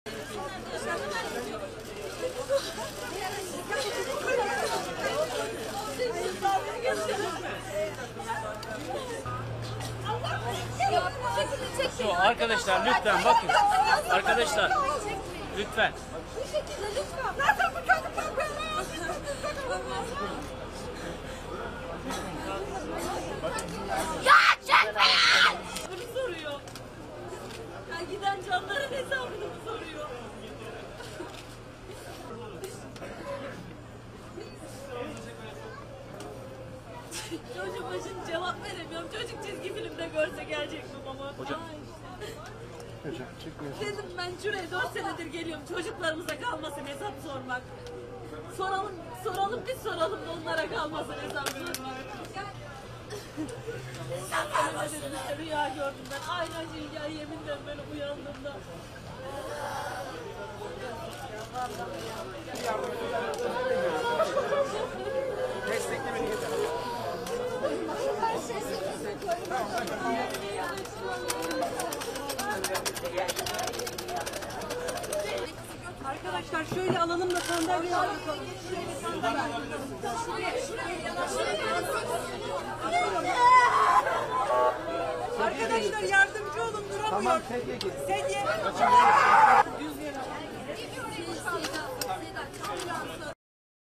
SOMA DAVASI'NDA KARAR AÇIKLANDI
301 işçinin hayatını kaybettiği Soma maden kazasının ceza davasında mahkeme heyeti tutuklu sanıklar için kararını açıkladıktan sonra salon bir anda karıştı.
  Mahkeme heyeti, haklarında bilinçli taksirle ölüme sebebiyet verme suçlamasıyla 301 kez 20-25 yıl ve 162 kez de 2 ile 6 yıl arası hapis cezası talep edilen tutuklu sanıklardan Soma Kömürleri A.Ş. Yönetim Kurulu Başkanı Can Gürkan’a taksirle birden fazla kişinin ölümüne sebebiyet verme suçundan 15 yıl hapis cezası ve 3 yıl boyunca maden ocağı işletmeme cezası verdi.